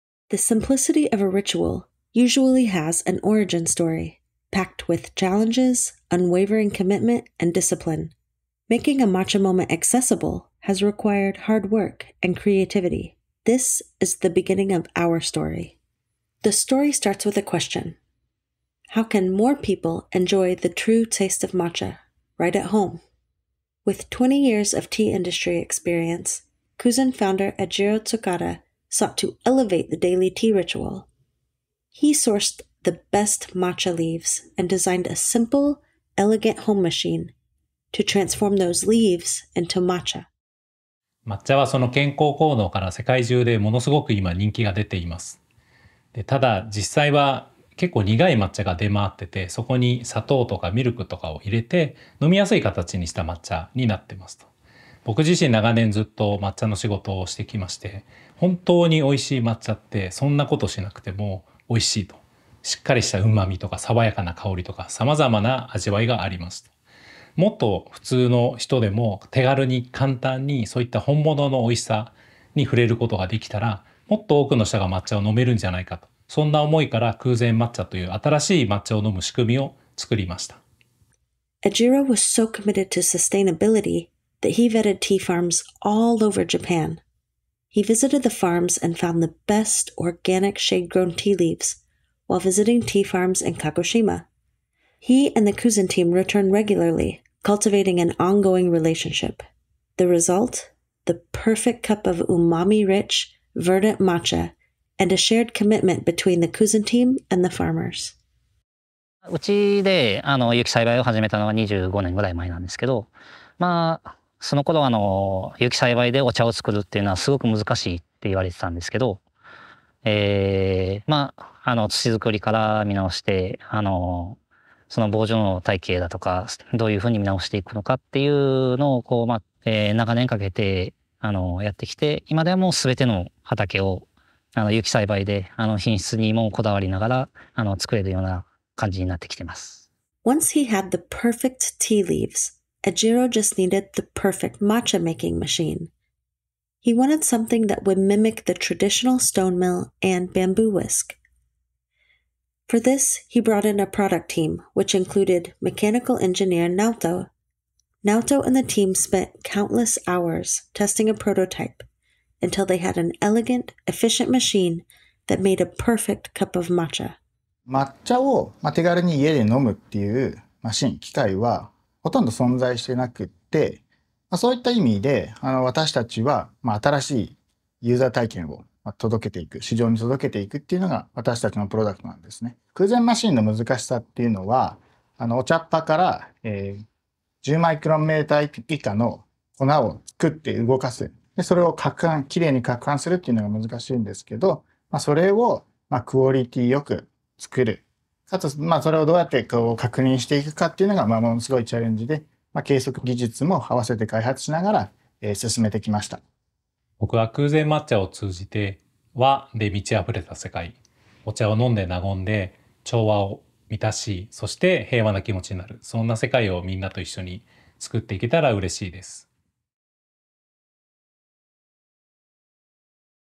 Cuzen Matcha - Our Story
I started Cuzen Matcha with my long-time friend, Oki Hatta, because we really love matcha, and we know freshly-ground organic matcha is better. Smells better, tastes better and feels better. The color is more vivid and the nutrients are preserved better. Drinking matcha with Cuzen is one healthy daily habit that’s easy. Fresh, organic leaves are sent to you, while grinding and whisking are made simple with the Matcha Maker. All you have to do is press a button and enjoy the moment.

We put together a video with footage of the tea farms we source from and interviews with our farming partner and mechanical engineer. We hope this will provide a deeper understanding of our commitment to accessible, sustainably-grown, fresh matcha.

— Eijiro Tsukada, Founder of Cuzen Matcha